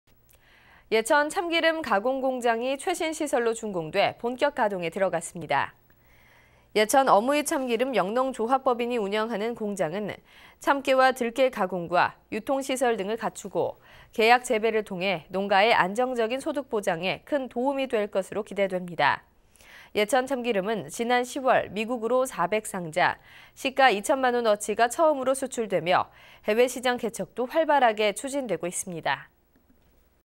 [안동MBC뉴스]예천 참기름 가공공장 준공(PM2시)
2015/11/25 10:09:38    작성자 : 김건엽

예천 참기름 가공공장이 최신시설로 준공돼
본격 가동에 들어갔습니다.
◀ＥＮＤ▶

예천 어무이 참기름 영농조합법인이 운영하는
공장은 참깨와 들깨 가공과 유통시설 등을
갖추고 계약재배를 통해 농가의 안정적인
소득 보장에 큰 도움이 될 것으로 기대됩니다.

예천 참기름은 지난 10월 미국으로 400상자,
시가 2천만원어치가 처음으로 수출되며
해외시장 개척도 활발하게 추진되고 있습니다.